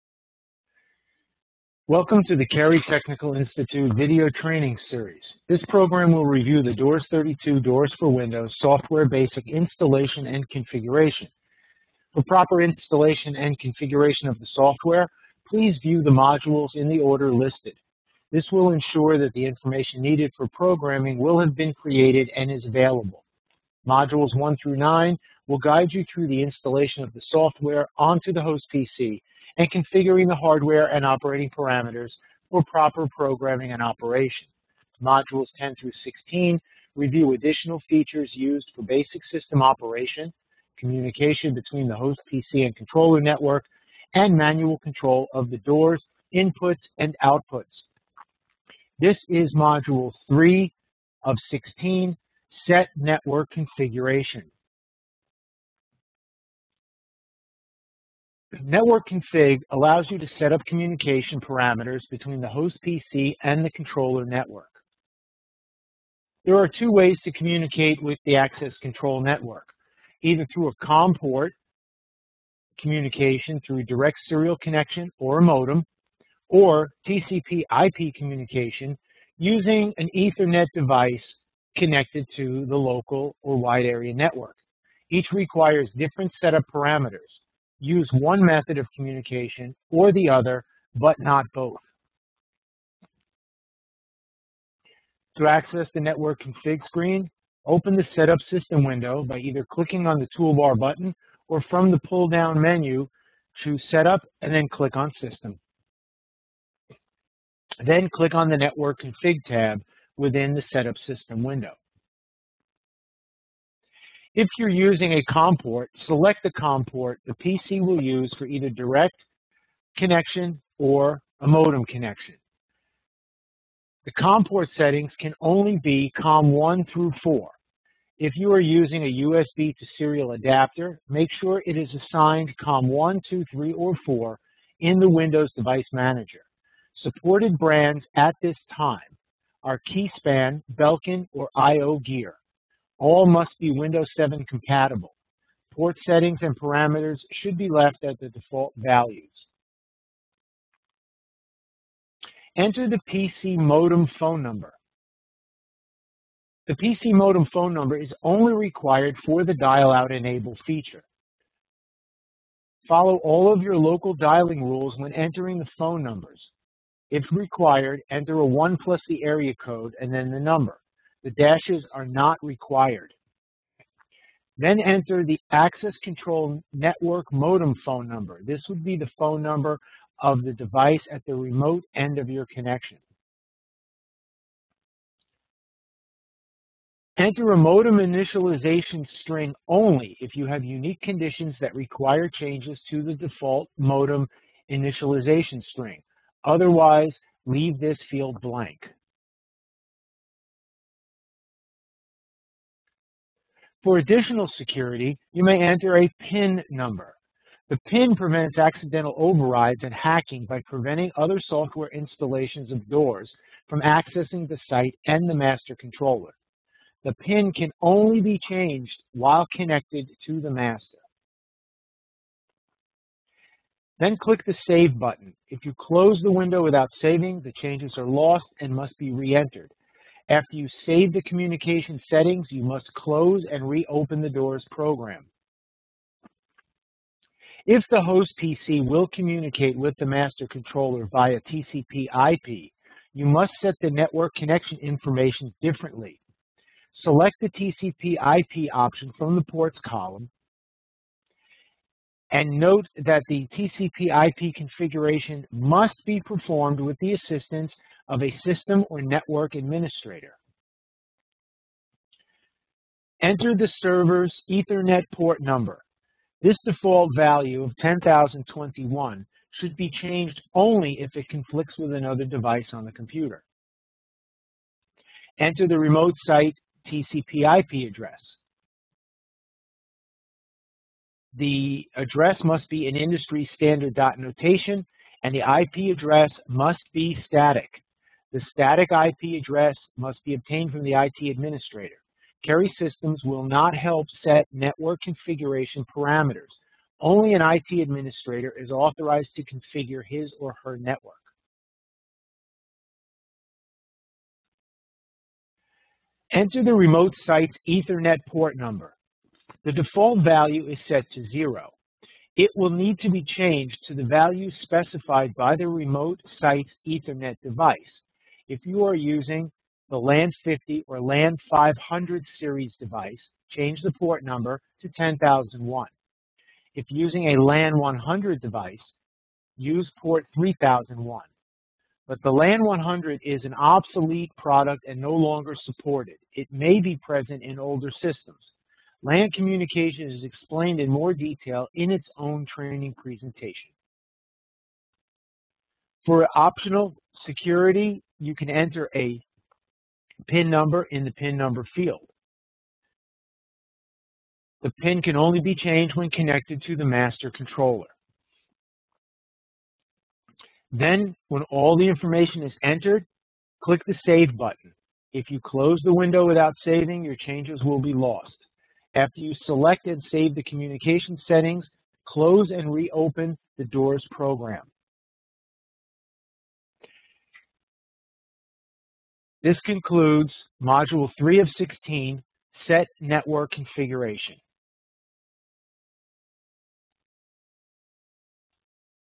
03 Set Network Configuration Doors32 SW Basic
Keri Systems Video Learning Series, This is Module 3 of 16 Modules. This Video covers the installation of Doors32 Software that controls PXL and Entraguard Series Controllers.